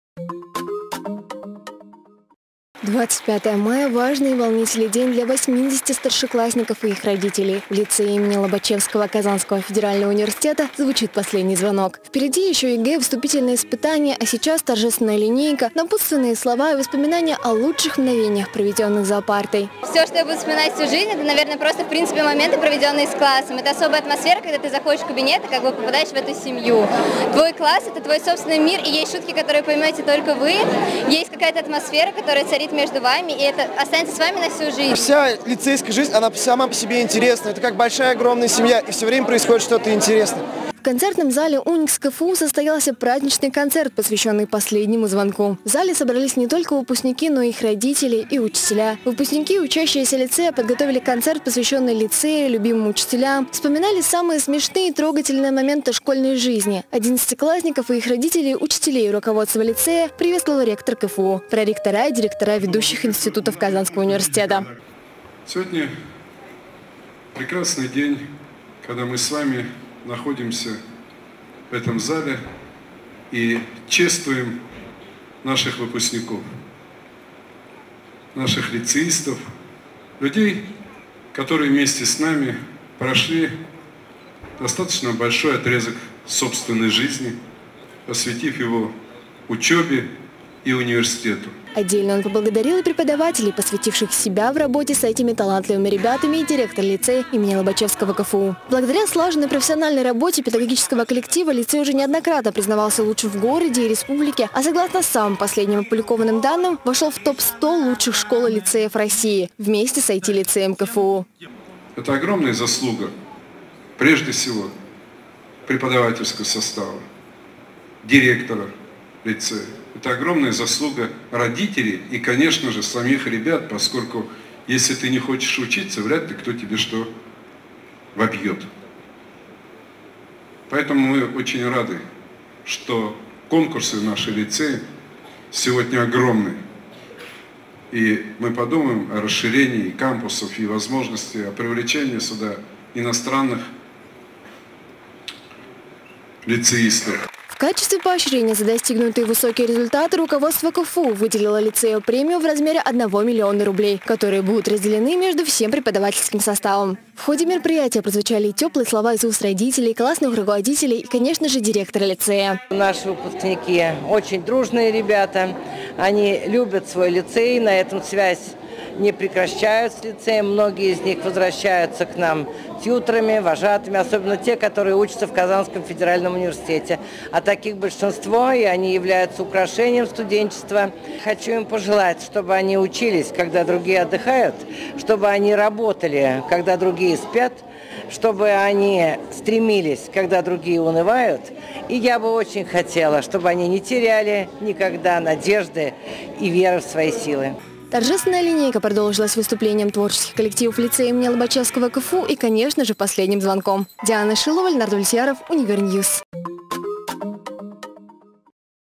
Последний звонок Лицея им. Н. И. Лобачевского КФУ
В Лицее имени Н.И. Лобачевского Казанского федерального университета прошло торжественное мероприятие, посвященное последнему звонку.